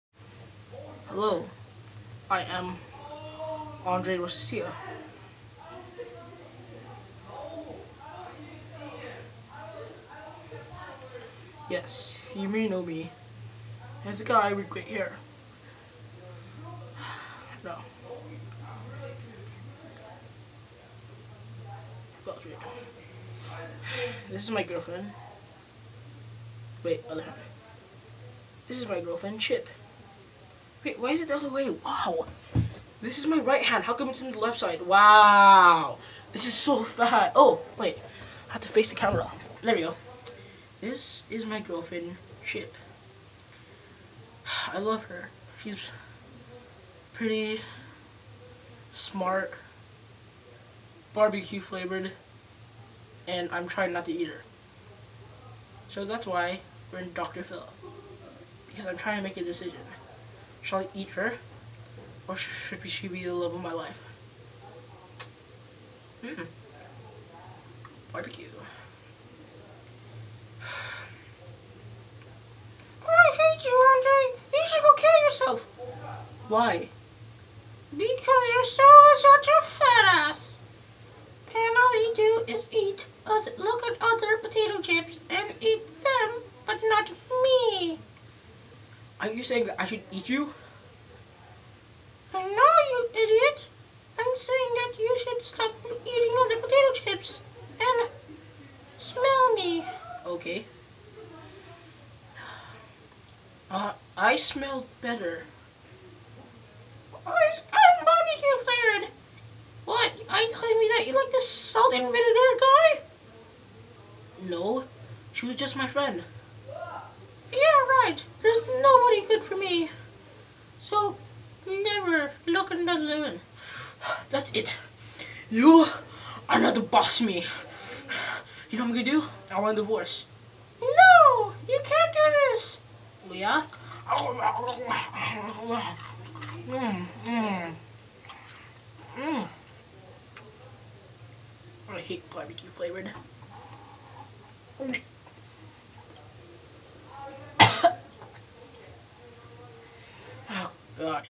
Andre and Chip: The Love Of His Life
Romance between a Fat Boy and a barbecue flavored chip can't last